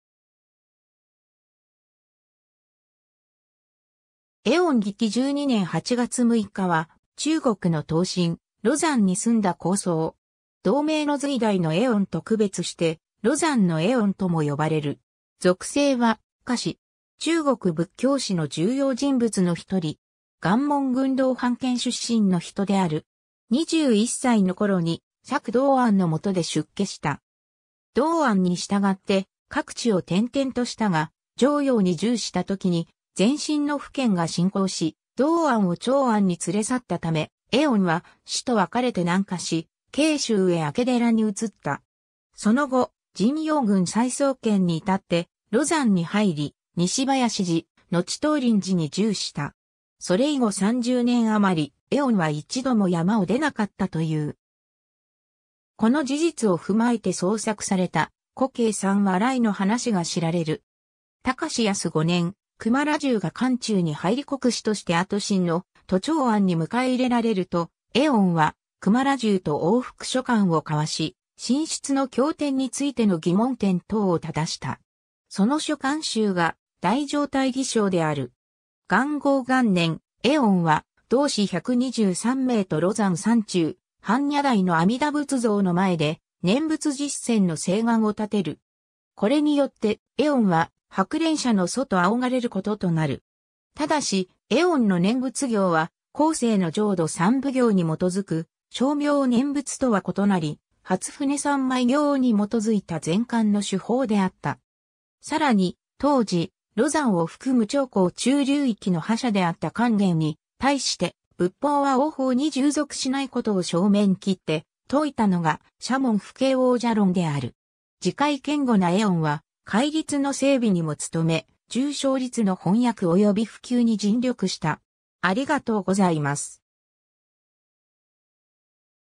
慧遠 (東晋)
慧遠（えおん、咸和9年（334年） - 義熙12年8月6日（416年9月13日））は、中国の東晋、廬山に住んだ高僧。
同名の隋代の慧遠と区別して廬山の慧遠とも呼ばれる。
俗姓は賈氏。
中国仏教史の重要人物の一人。
 雁門郡楼煩県（山西省忻州市寧武県）出身の人である。
21歳の頃に釈道安の元で出家した。
道安に随って各地を転々としたが、襄陽に住した時に前秦の苻堅が侵攻し、道安を長安に連れ去ったため、慧遠は師と別れて南下し、荊州上明寺に移った。
 その後、尋陽郡柴桑県に至って廬山に入り、西林寺、のち東林寺に住した。
それ以後30年余り、慧遠は一度も山を出なかったという。
この事実を踏まえて創作された「虎渓三笑」の話が知られる。
 隆安5年（401年）、鳩摩羅什が関中に入り国師として後秦の都長安に迎え入れられると、慧遠は鳩摩羅什と往復書簡を交わし、新出の経典についての疑問点等をただした。
その書簡集が『大乗大義章』である。
 元興元年（402年）、慧遠は同志123名と廬山山中・般若台の阿弥陀仏像の前で、念仏実践の誓願を立てる。
これによって、慧遠は白蓮社の祖と仰がれることとなる。
ただし、慧遠の念仏行は、後世の浄土三部経に基づく称名念仏とは異なり、『般舟三昧経』に基づいた禅観の修法であった。
 さらに、当時、廬山を含む長江中流域の覇者であった桓玄に対して、仏法は王法に従属しないことを正面きって説いたのが『沙門不敬王者論』である。
 持戒堅固な慧遠は戒律の整備にも努め、『十誦律』の翻訳及び普及に尽力した。